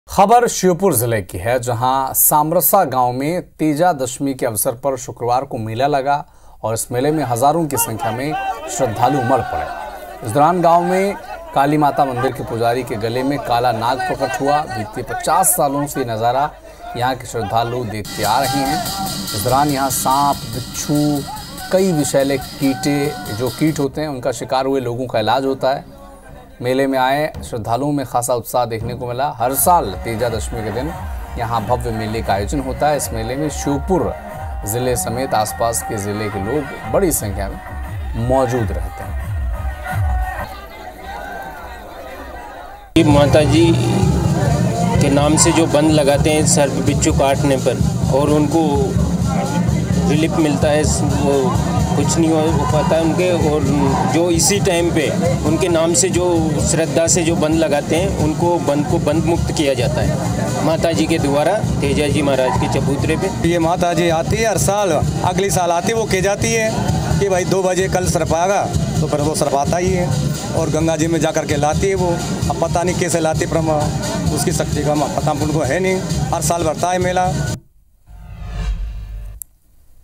Sheopur News : तेजा दशमी पर सामरसा गांव में मेंला का आयोजन। मेले में उमड़े हजारों श्रद्धालु
IBC24, Sheopur News , तेजा दशमी पर सामरसा गांव में मेंला का आयोजन, मेले में उमड़े हजारों श्रद्धालु